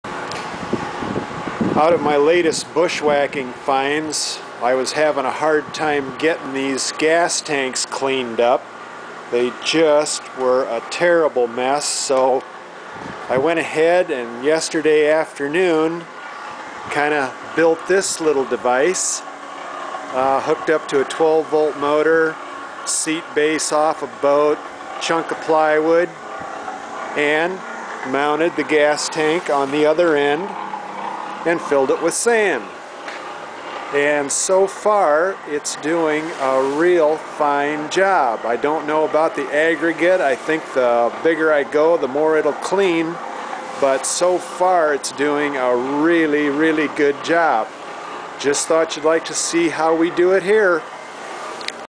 Movie cleaning gas tanks.mpg
After many years of setting the fuel tanks on these sleds were covered with rust inside so with a little time and used parts i whipped up a tumbler. All that's in the tank is sand so it's not so agressive and it's doing a sweet job of cleaning them up!